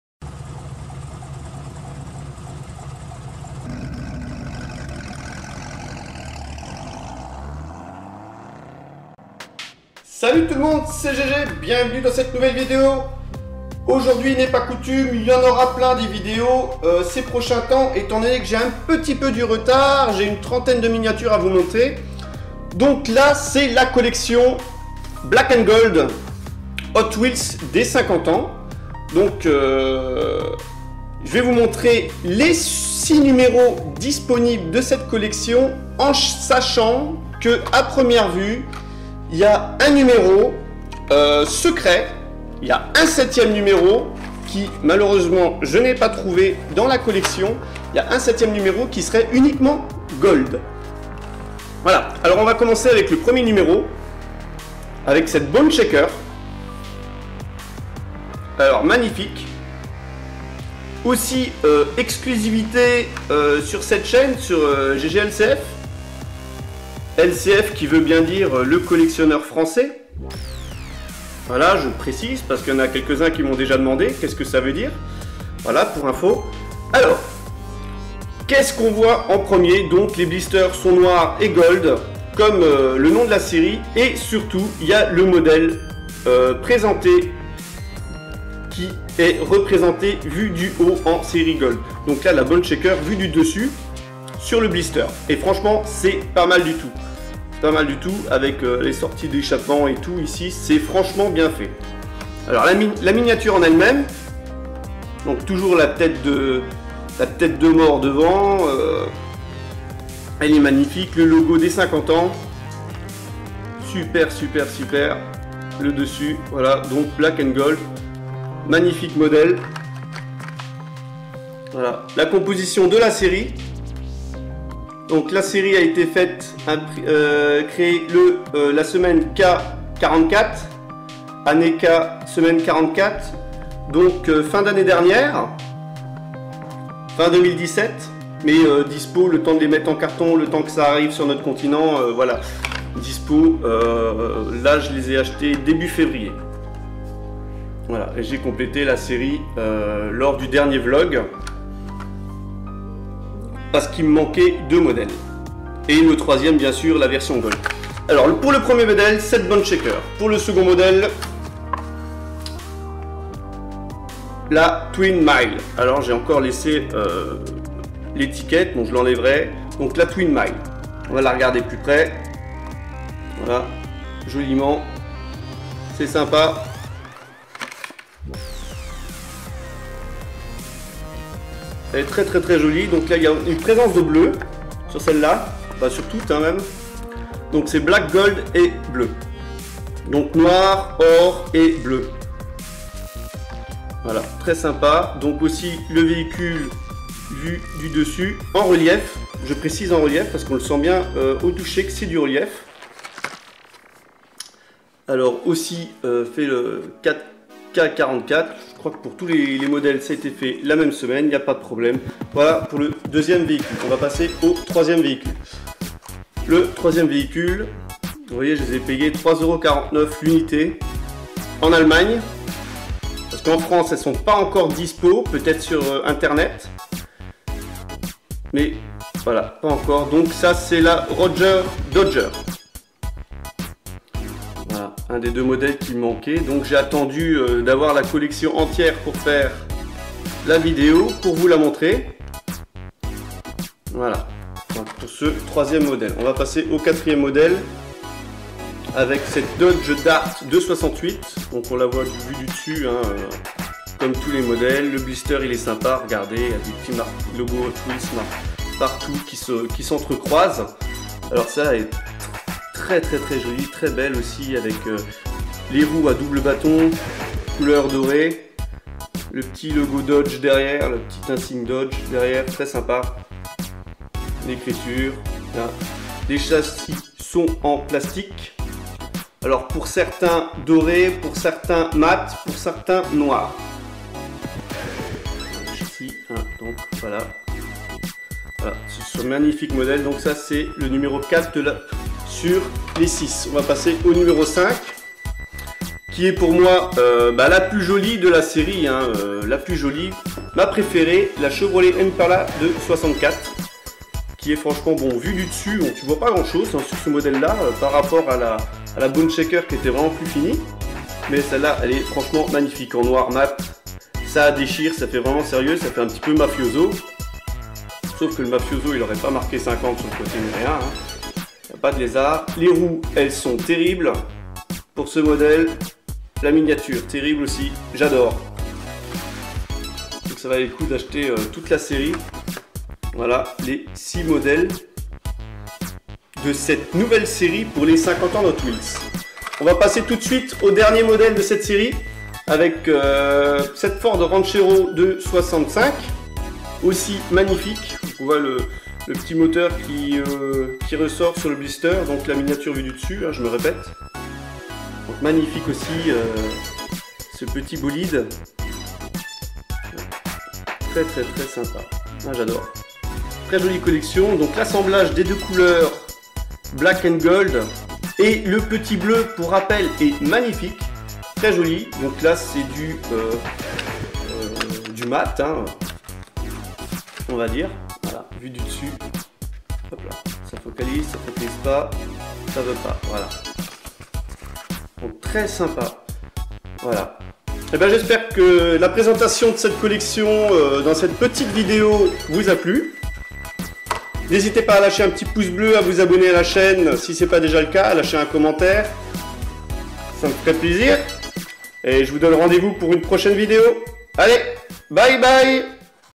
Black and Gold Collection Hotwheels 50 ans
Présentation de la collection Black and Gold Hotwheels , collection sortie pour les 50 ans du fabricant .

Bon visionnage ;)